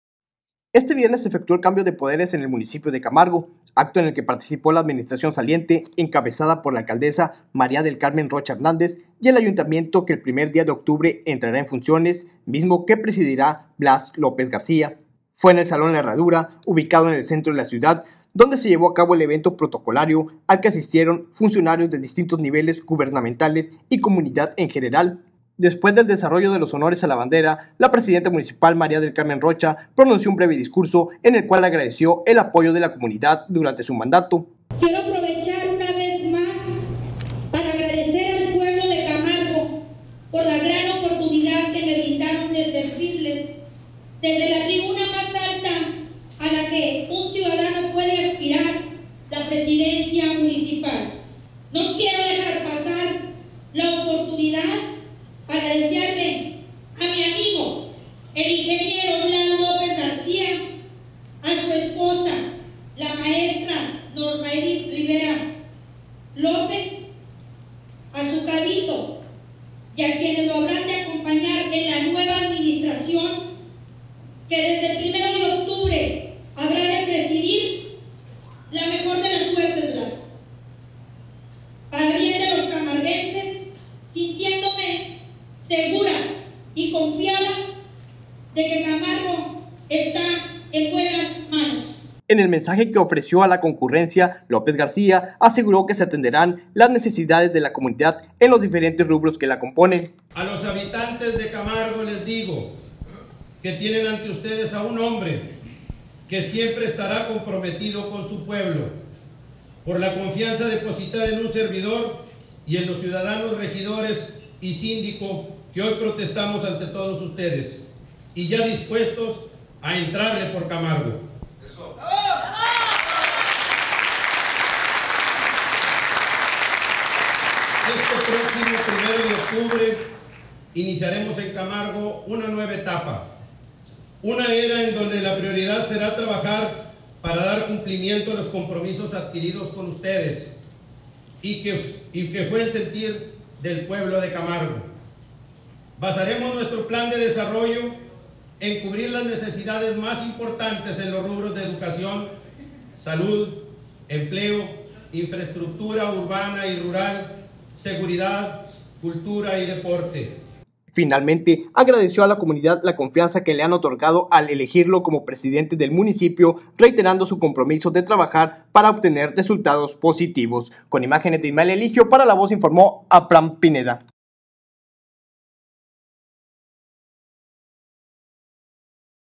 Hora Cero - Blas López Alcalde
Este viernes se llevó a cabo la toma de protesta del nuevo ayuntamiento de Camargo, Tamaulipas.